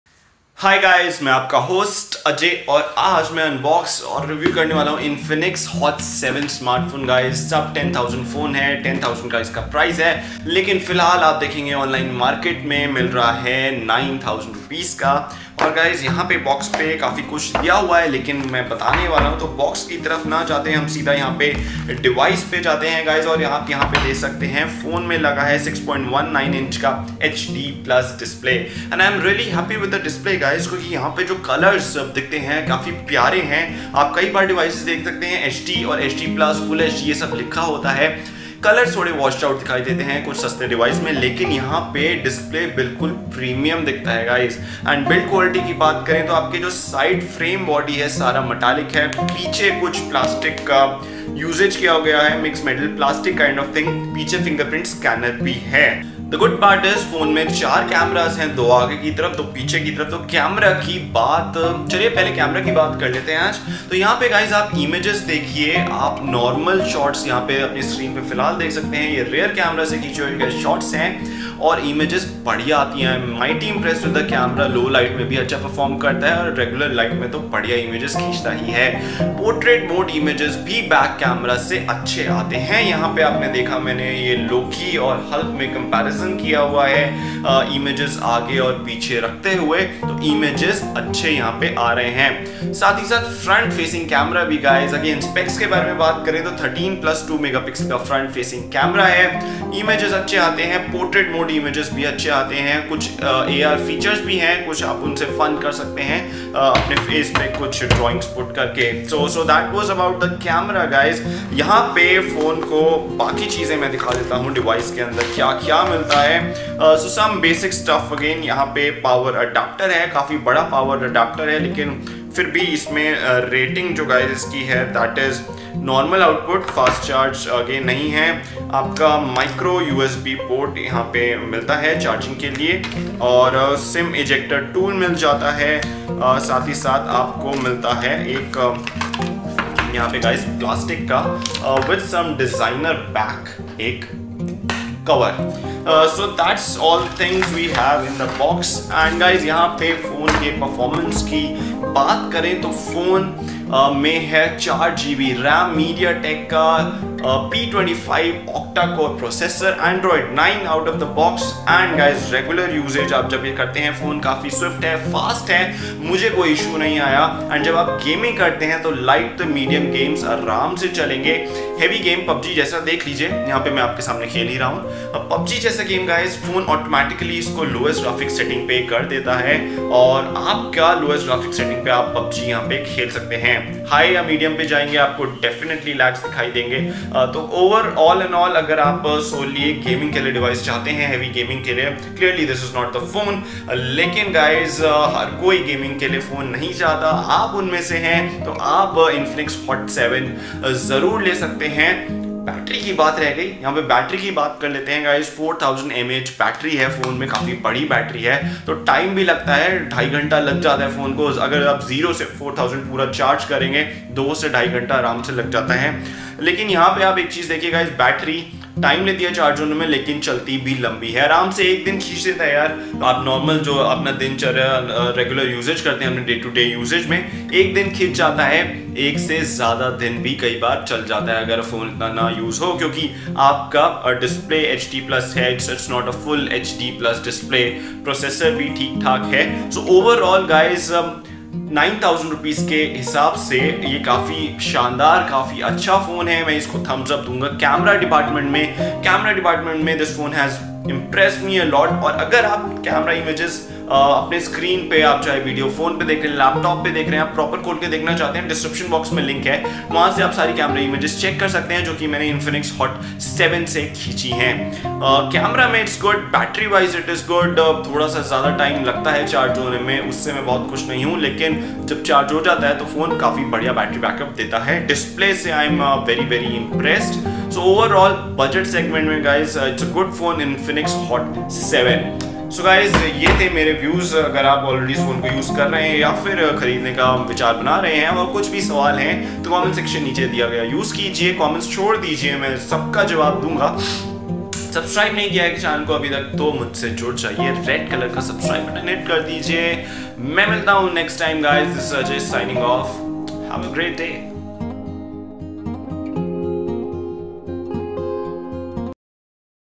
Infinix Hot 7 Unboxing, Review And Camera Images
The Infinix Hot 7 is a budget phone with a total of 4 cameras. the phone can click very good images.